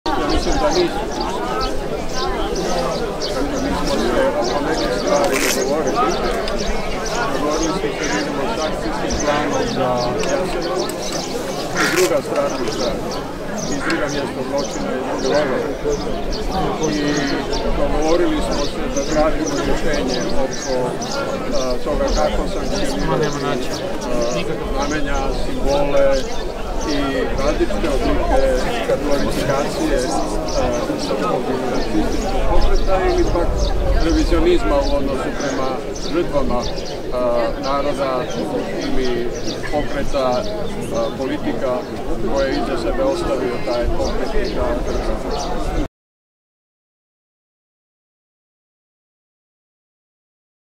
Pupovac dao izjavu povodom 77. obljetnice proboja zatočenika Jasenovca
Povodom 77. obljetnice proboja posljednjih zatočenika ustaškog logora Jasenovac, izaslanstva Predsjednika, Sabora, Vlade, parlamentarnih stranaka, te predstavnici Saveza antifašističkih boraca i antifašista, Srpskog narodnog vijeća kao i manjina, odali su počast svim stradalima u tom logoru polaganjem cvijeća i kamenčića kod Spomen obilježja Kameni cvijet.